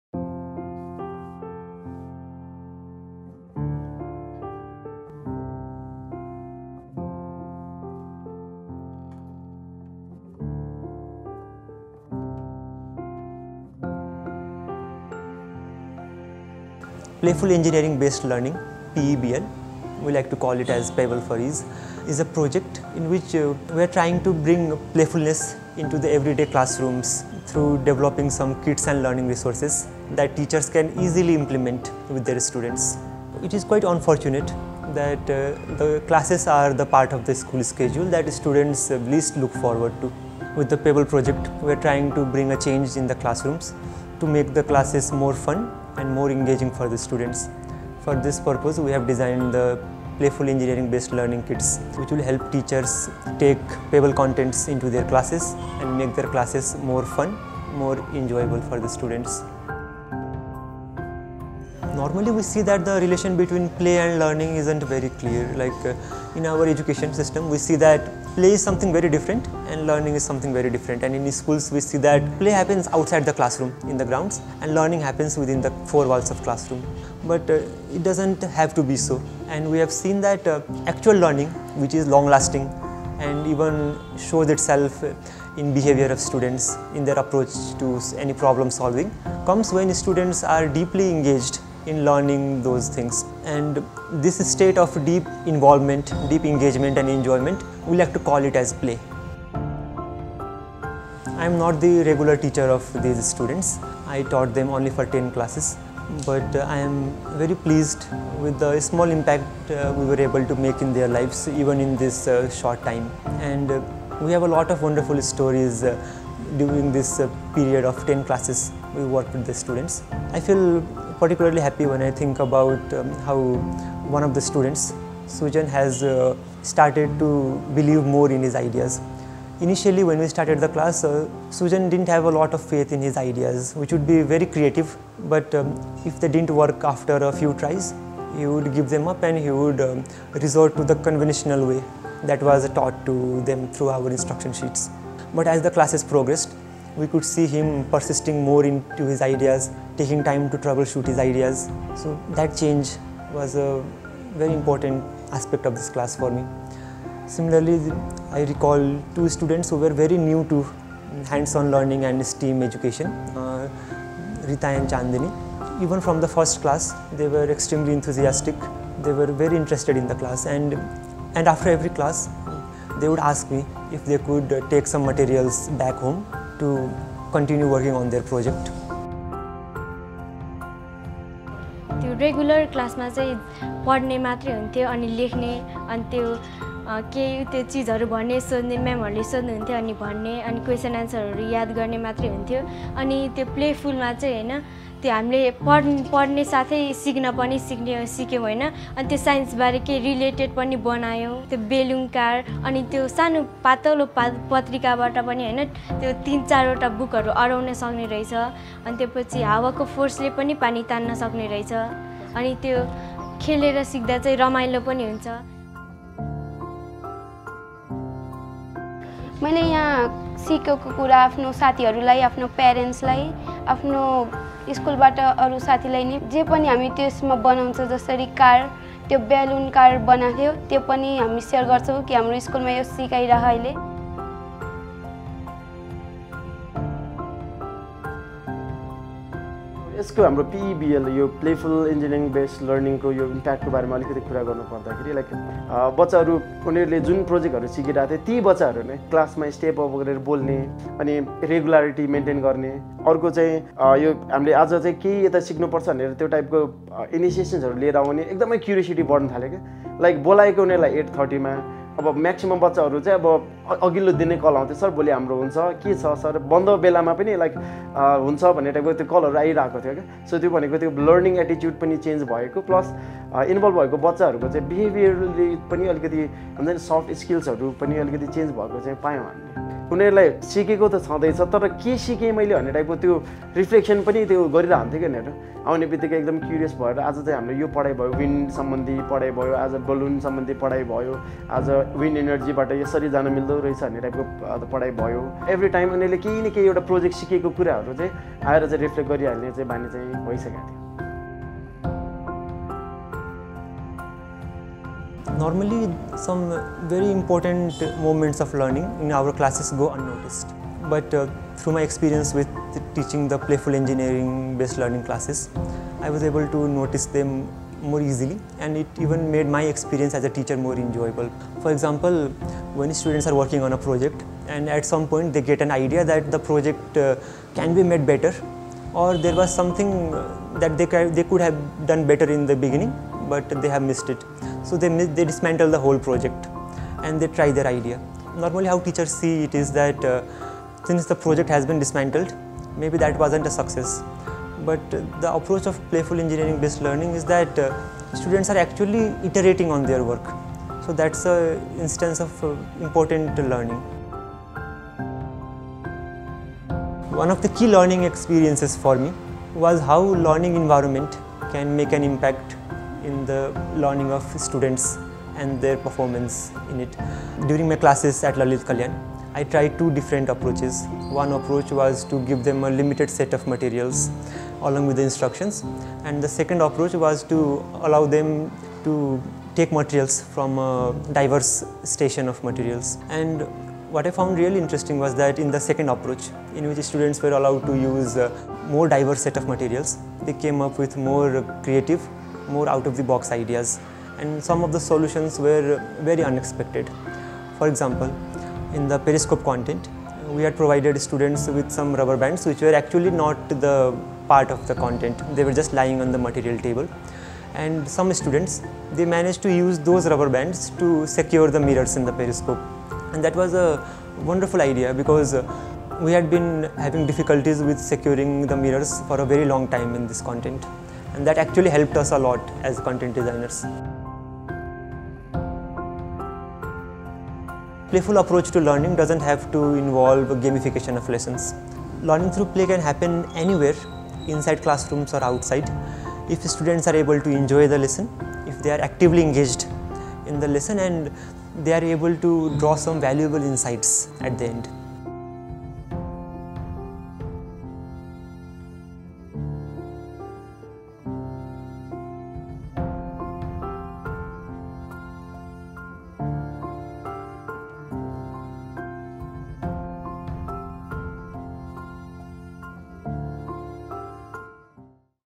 PEBL sessions at Lalit Kalyan Kendra School
With the Playful Engineering-Based Learning (PEBL) project, Karkhana Samuha aims to implement a learning through play approach for students in everyday classrooms. Our PEBL kits and learning resources are designed to engage students in their own learning through playful experiences. PEBL sessions facilitated by trained teachers are hands-on, active, and fun.
In this video, educators - Sameer Prasai and Rajendra Kumar Deshar, along with some students share their experiences of being a part of the PEBL sessions at Lalit Kalyan School.

Video Credits: Pramin Manandhar & Robic Upadhyaya